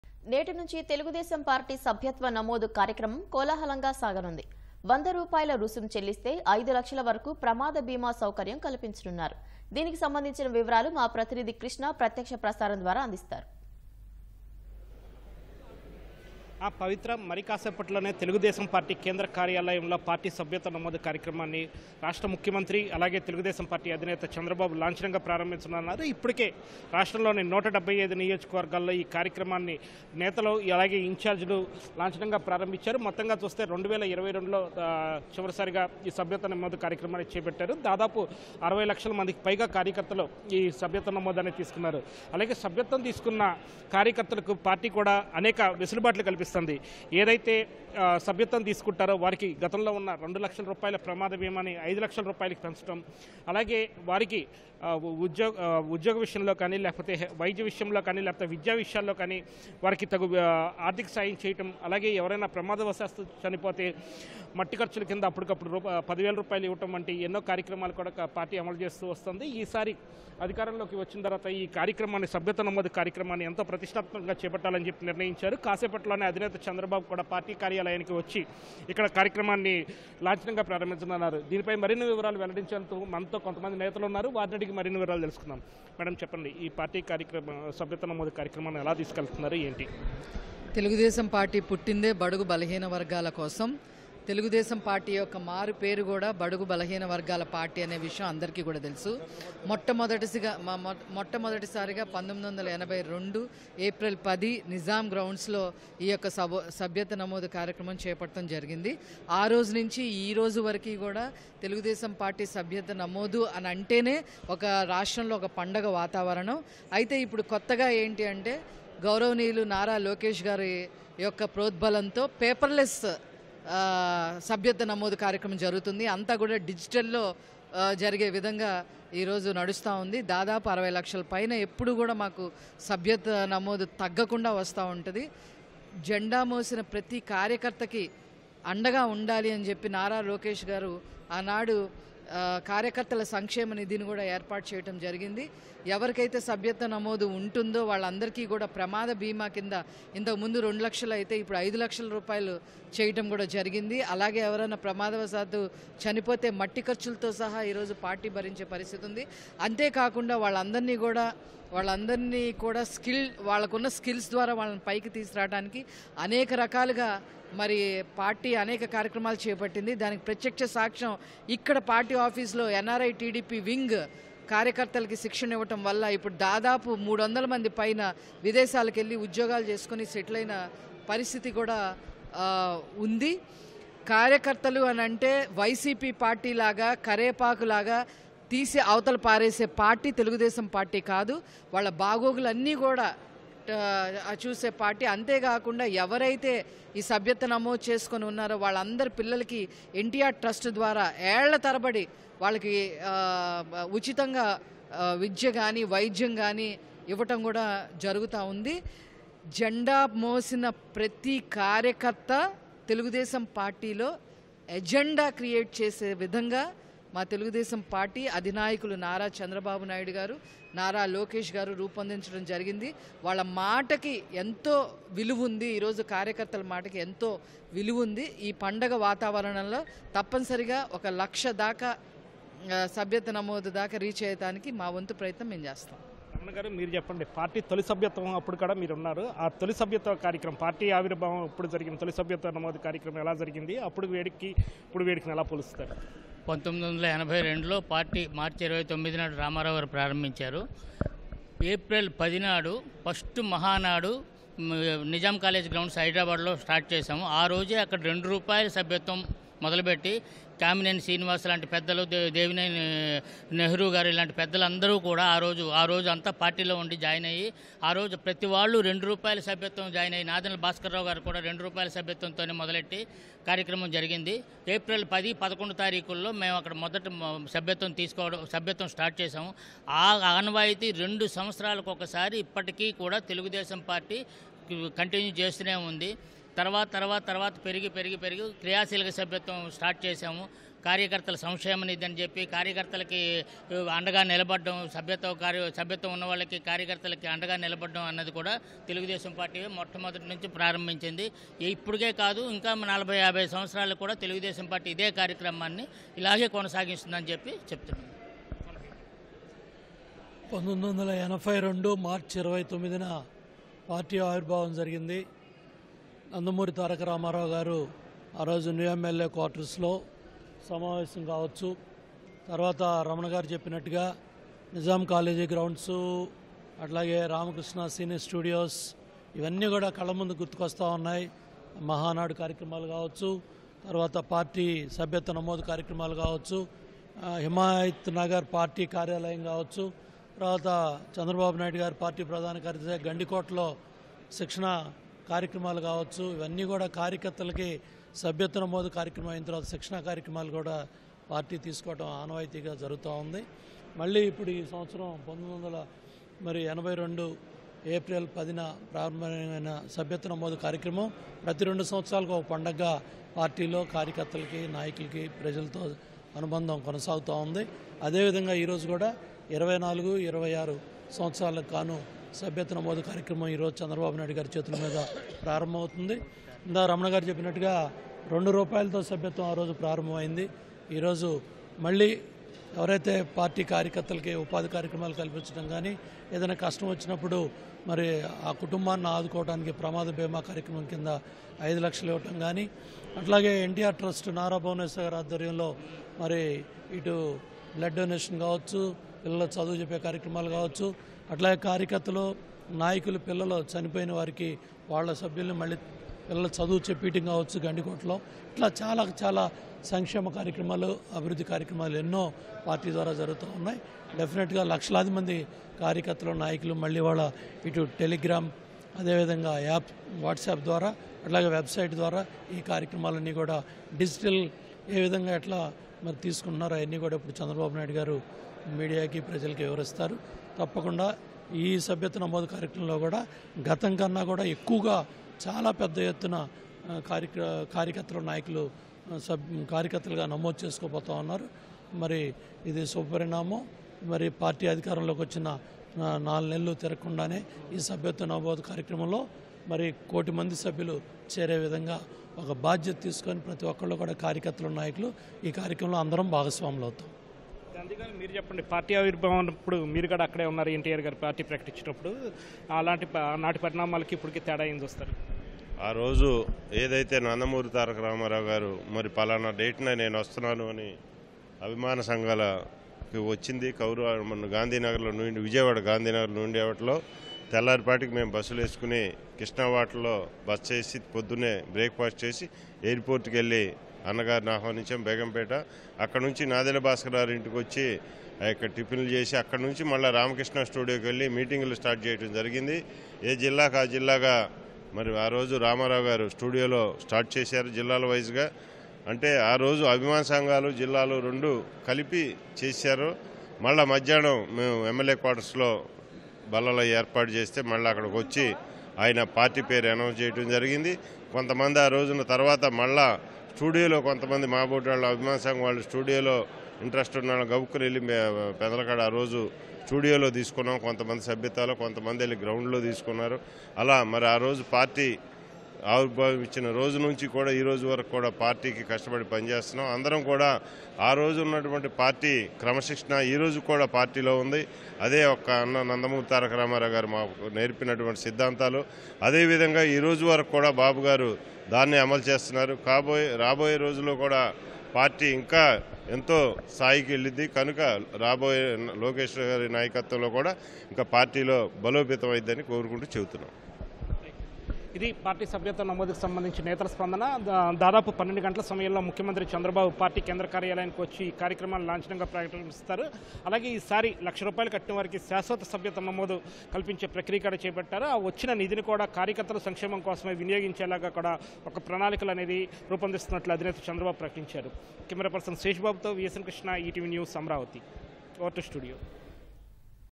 టీడీపీ సభ్యత్వ నమోదు కార్యక్రమం | TDP Leaders Interview | TDP Membership Registration Program
నేటి నుంచి తెలుగుదేశం పార్టీ సభ్యత్వ నమోదు కార్యక్రమం కోలాహలంగా సాగనుంది. వంద రూపాయల రుసుం చెల్లిస్తే... 5 లక్షల వరకు ప్రమాద బీమా సౌకర్యం కల్పించనున్నారు.